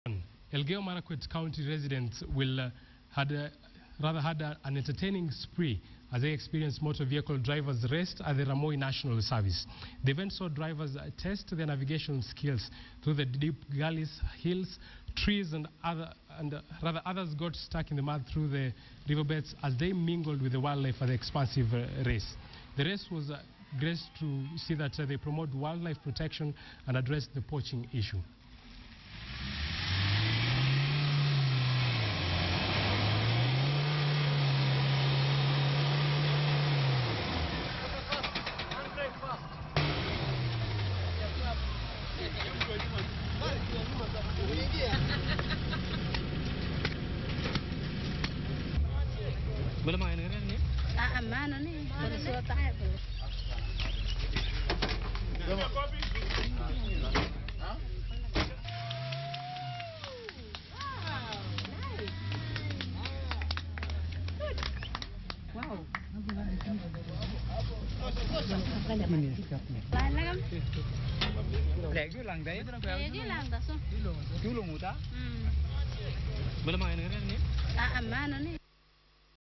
Elgeiyo Marakwet residents experience motor vehicle drivers race at Rimoi National game reserve
Elgeiyo Marakwet county residents had an entertaining spree as they expereinced motor vehicle drivers raced at Rimoi National game reserve
The event saw drivers test their navigating skills through the deep gulleys, hills and trees. Others were stuck in mud driving through river beds as they mingled with wildlife at the expansive reserve.